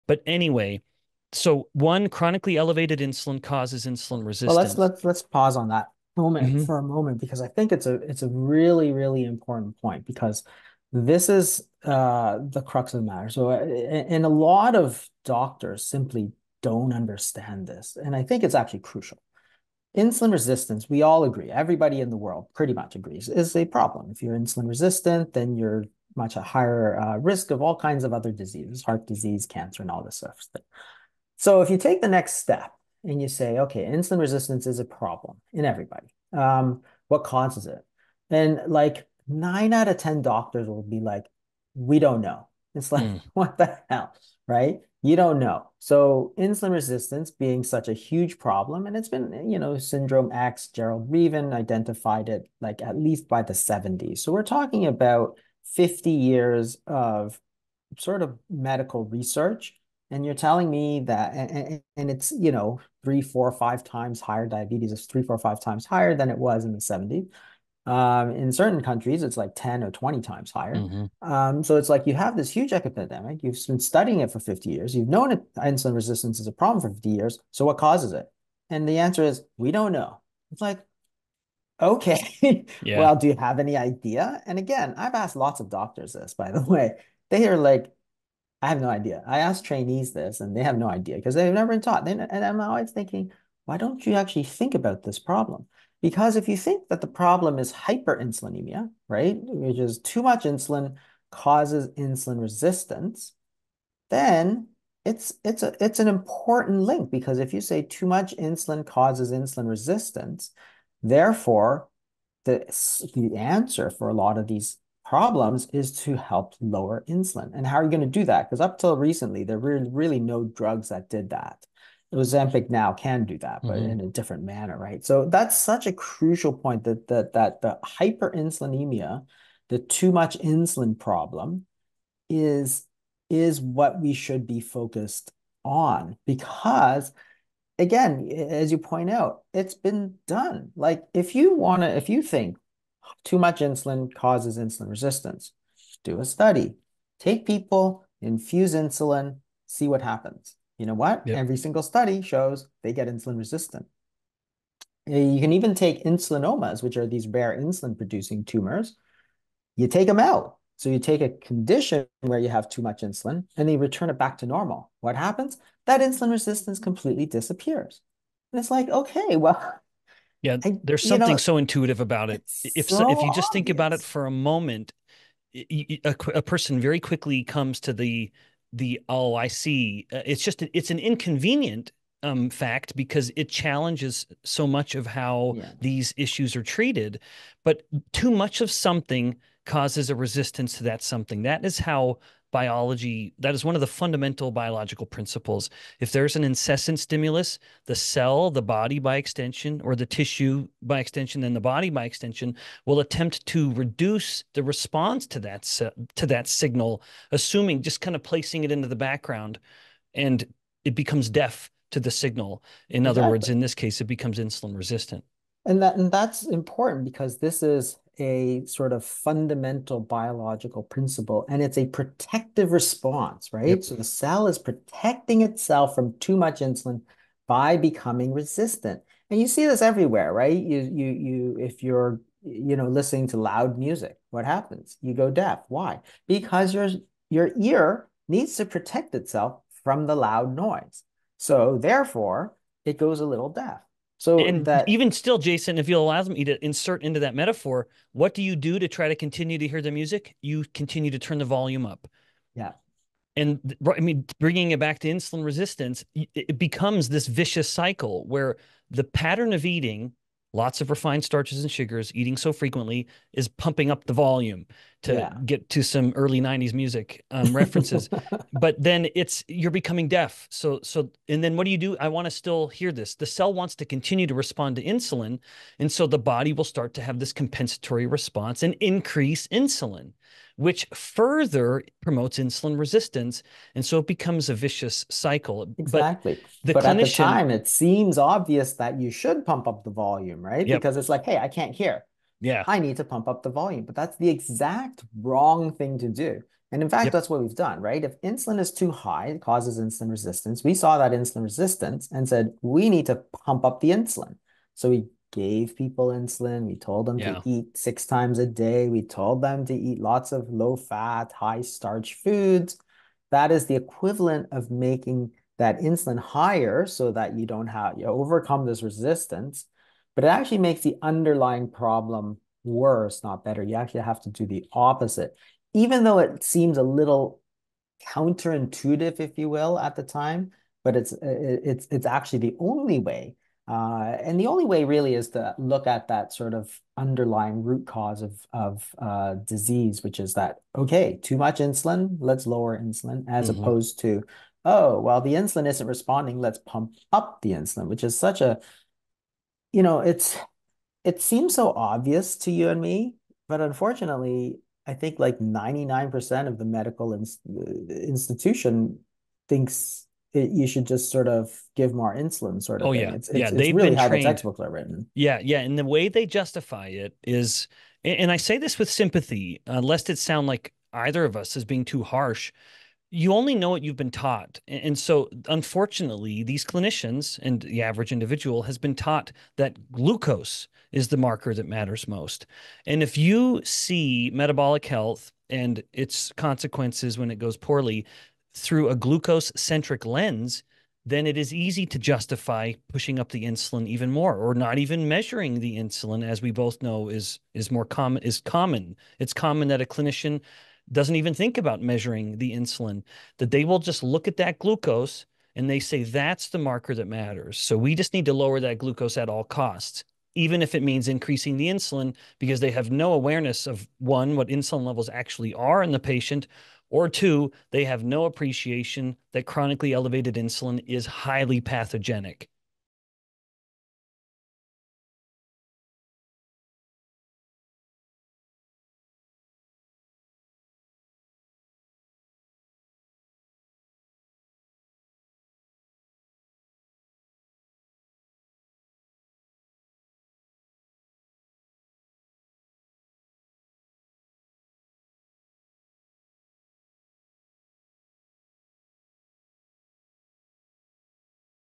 BEN BIKMAN f4 | 90% DOCTORS: IGNORANT of IR CAUSE …they see METABOLIC HEALTH thru GLUCOSE-ONLY LENS
presents episode 2437 | DR BEN BIKMAN w/Jason Fung

IR is a problem...everyone agrees…
-much higher risk of many, many diseases
What is the cause of IR?
-90% of doctors Do Not Know
For 50 years it has been identified, and known that it causes
10X more diabetes, and other diseases
-it is not taught in med school
If it is due to hyperinsulinemia, work on
Lowering the Insulin

Too much insulin is one of the key drivers of IR
-shown in every study: more insulin leads to IR
-take out insulin-producing tumors...IR goes away
But, it is an inconvenient fact because it challenges dogma
Fundamental biological principle:
-too much of something causes a resistance to it
-cells, tissues, the body will stop listening to
  a constant chronic signal
-it is a protective response
Example: ultra loud music? over time, ear
will become slightly deaf for protection

What do we do when this happens?
-we turn the volume UP
Becomes a vicious cycle: insulin not working, add more insulin
-eat high carb 6X a day, pumps up insulin, prompts us to
  continue eating higher carbs, more often
-you are becoming more & more IR, but body
 is saying: more & more insulin!
This is the typical response of the medical
community across the world
Actually: Must Do The Opposite

99% of Current Medical Community will Pump Up Insulin…
-justify it because their Training was: Glucose is culprit
If you see metabolic health trough a glucose-centric lens
then: easy to
1-push up insulin to reduce glucose , or
2-not even measure insulin at all

We so not use corruptible, epidemiological survey research as causal science.